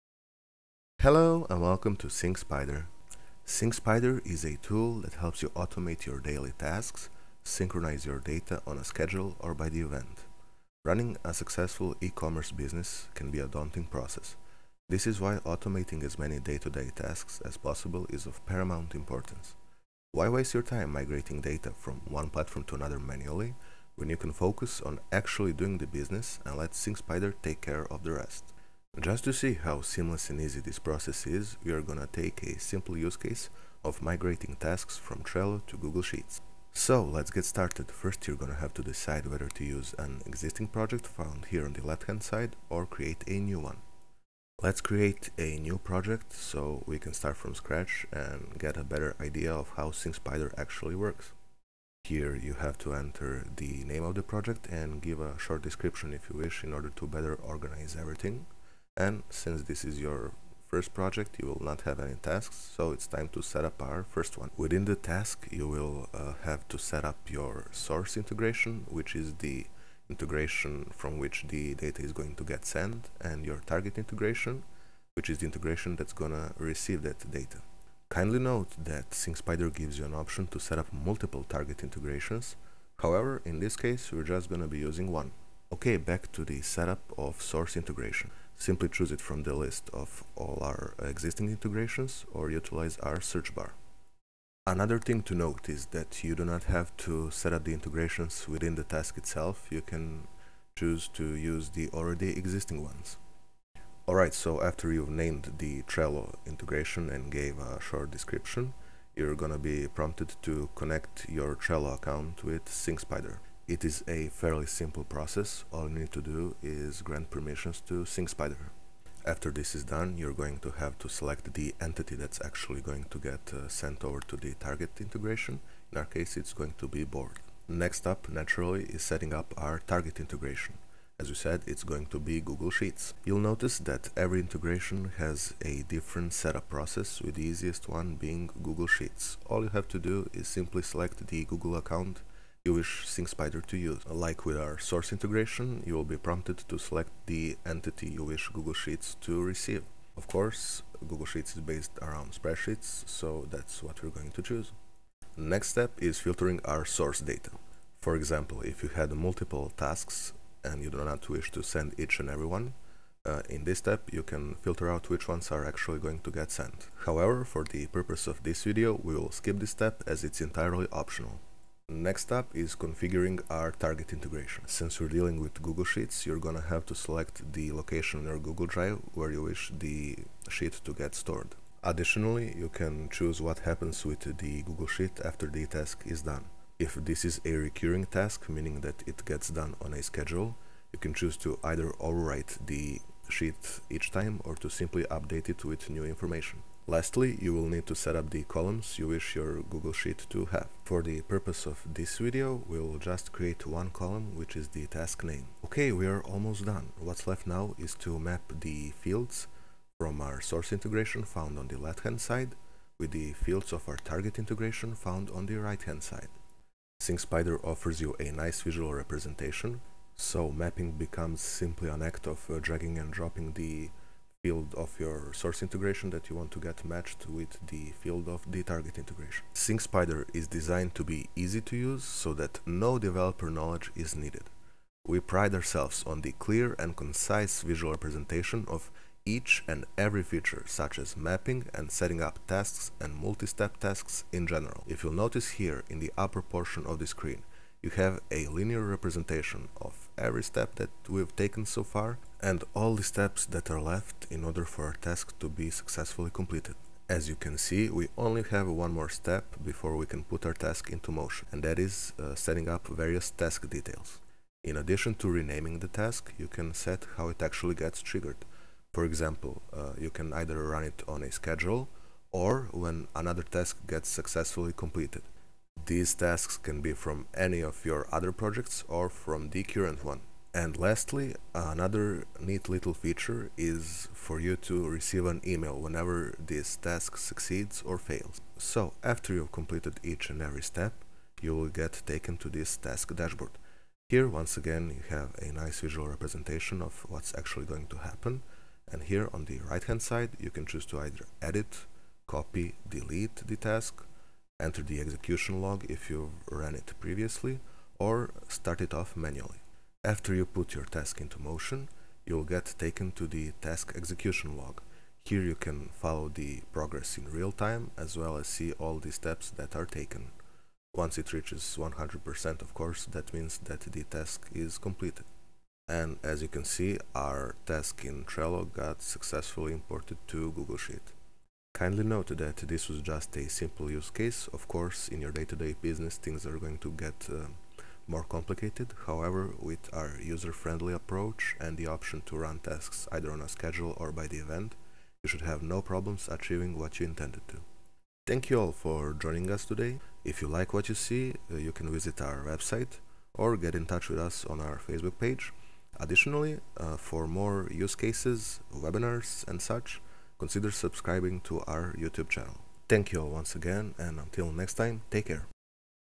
SyncSpider General Features Overview
Welcome to SyncSpider! In this video, we will go over SyncSpider's main features and advantages such as:
•  Visual representation of all the steps within a task
•  Intuitive process of Mapping
•  Multiple ways of starting the task such as on a schedule or after another task
•  Clear and concise Project Dashboard

HAVEN’T HEARD OF SYNCSPIDER BEFORE?

SyncSpider is a tool that helps you automate your daily tasks, synchronize your data on a schedule or by the event.